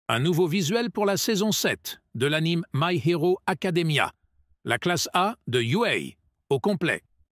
Révélation : La Saison 7 de My Hero Academia dévoile un nouveau visuel époustouflant !"
"Découvrez en exclusivité le tout nouveau visuel captivant de la Saison 7 de My Hero Academia, mettant en avant la Classe A de Yuei au complet ! Plongez dans l'univers épique des héros et des super-pouvoirs avec cette révélation sensationnelle !"